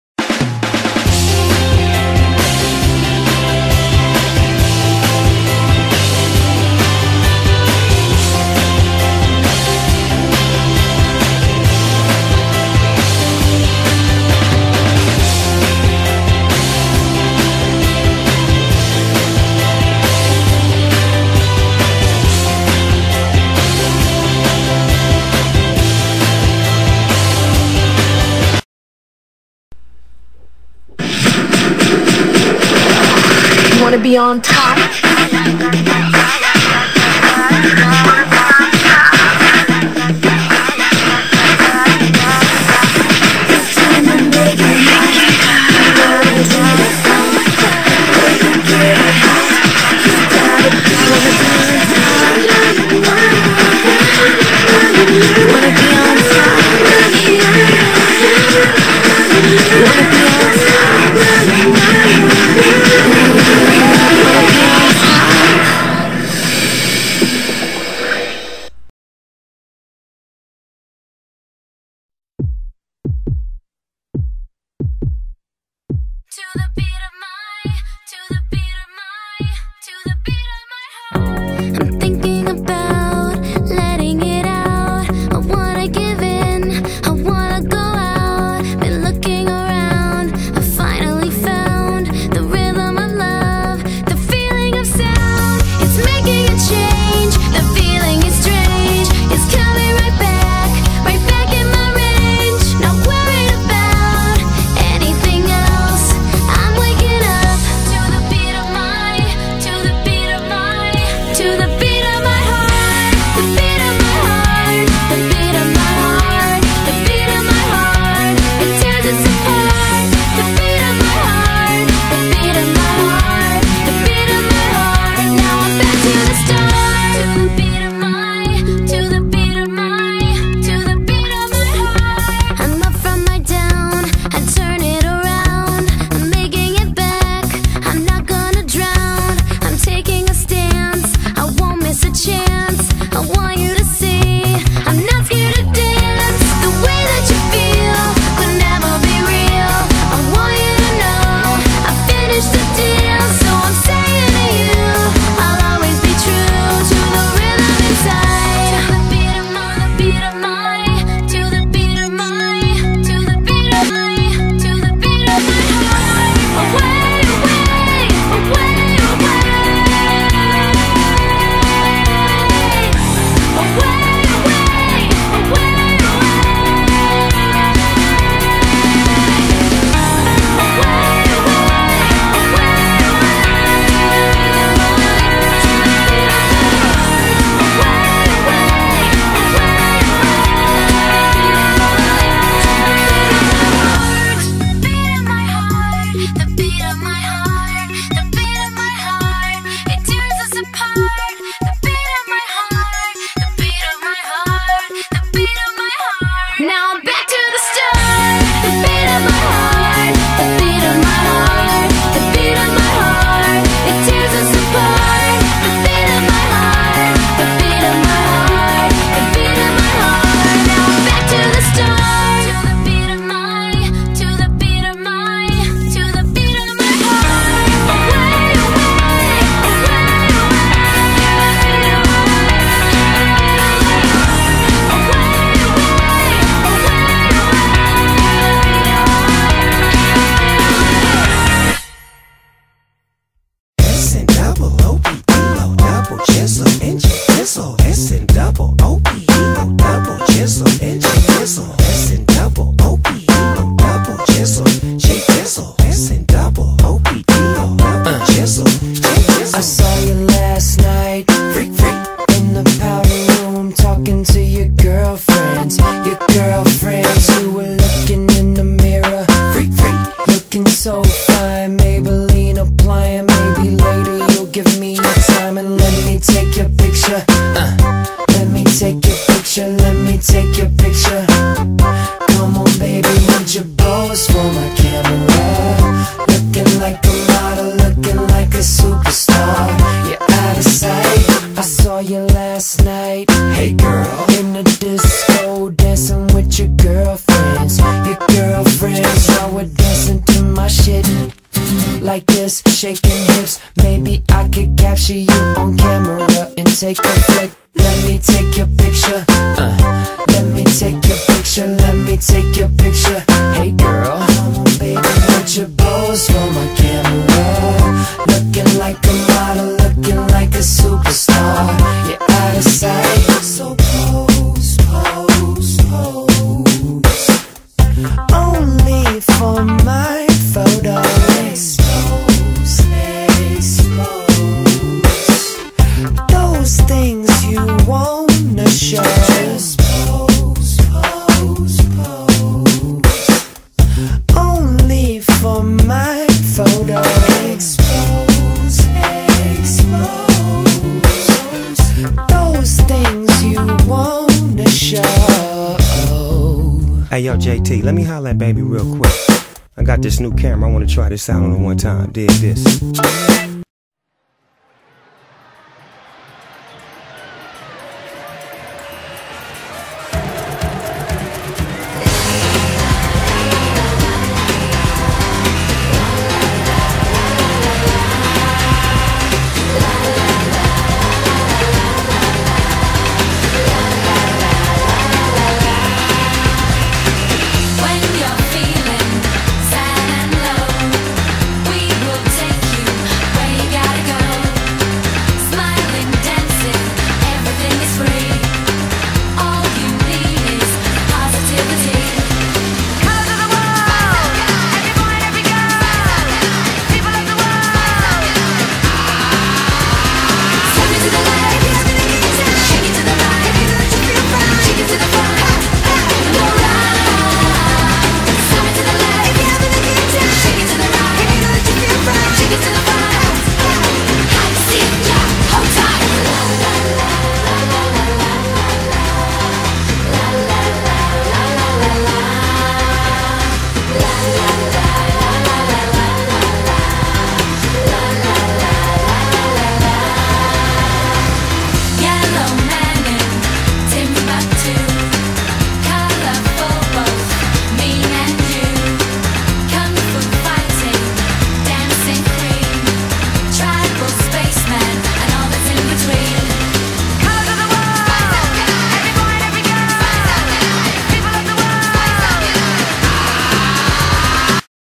Cycle 2 Episode 4 part 1
Her is episode 4! The girls have alot of drama in the house. Vote for your Covergirl!